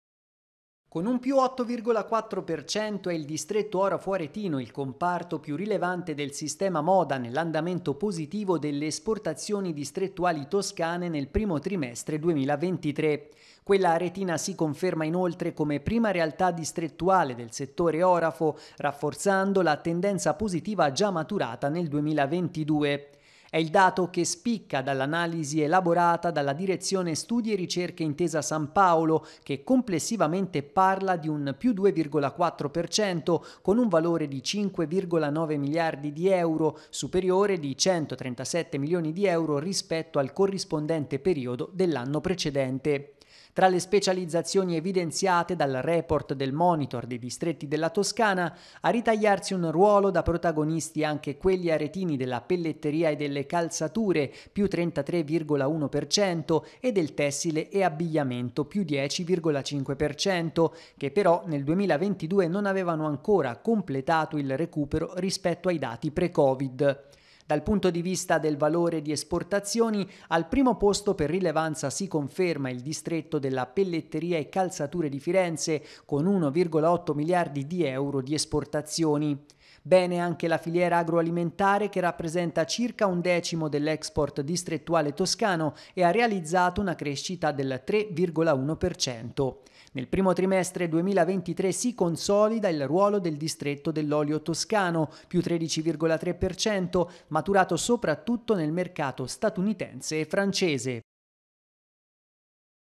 Export toscano, distretti aretini in evidenza
Quello toscano continua a rappresentare un sistema produttivo distrettuale con una spiccata vocazione all’export. Lo conferma lo studio svolto dalla Direzione Studi e Ricerche di Intesa Sanpaolo per il primo trimestre 2023. Distretto orafo aretino in evidenza.